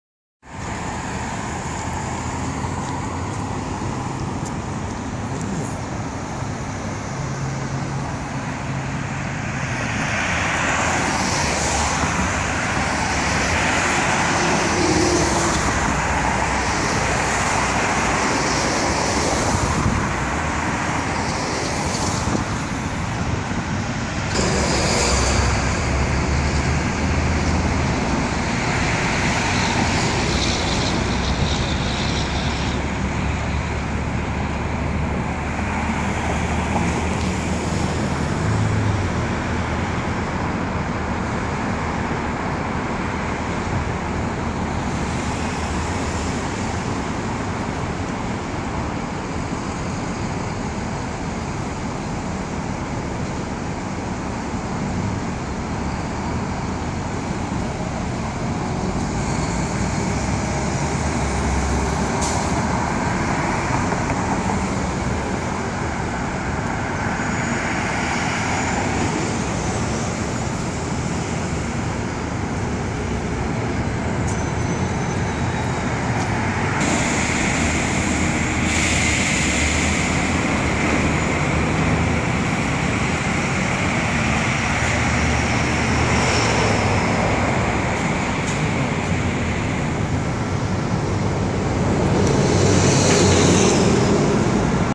GLA45 AMG Acceleration 20150505 072516
Replacing the S55 AMG V8 power sedan, it's owner upgrades to Mercedes Benz GLA45 AMG. A turbocharged engine w 355 Hp w 332 Lb-ft of torque. Waited for a green light, then filmed it turning, video nxt. Share on Pinterest, Tumblr.